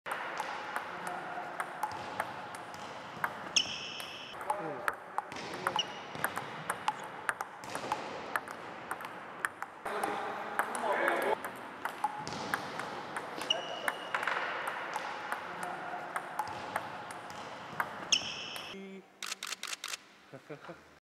Alonso Ping Pong @ Lanzarote Training Camp in January 2012
Fernando Alonso performs another magic trick. Now it's better to put children away from screen.